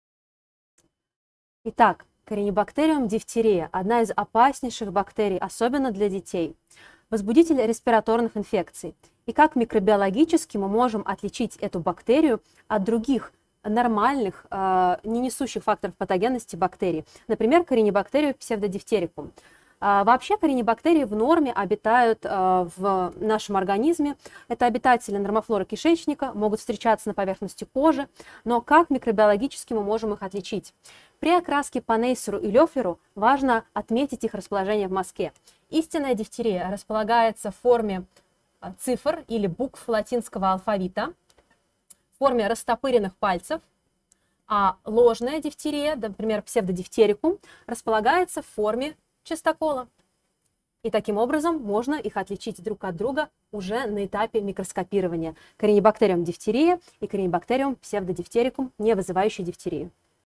SmartLearn: Татьяна Колыганова "Дифтерия"
Лектор: Татьяна Игоревна Колыганова, к.м.н. ассистент кафедры микробиологии, вирусологии и иммунологии

Скоро сдавать экзамен по микробиологии?
Получи бесплатный демо-доступ к самым важным темам по ссылке